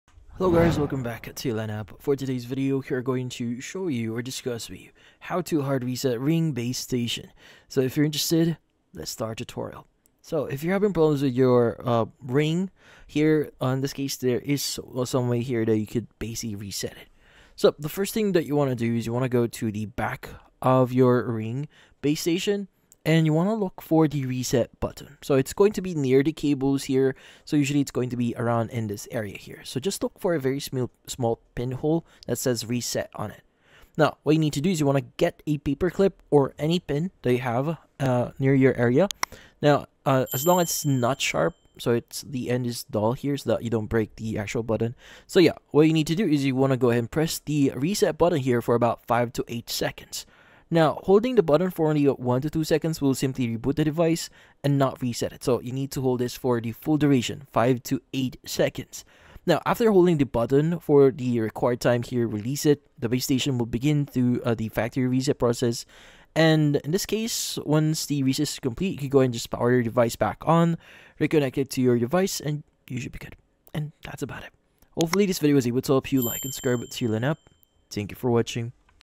How To Hard Reset Ring Base Station (2025)
If you need to hard reset your Ring Base Station, then this tutorial will be perfect for you!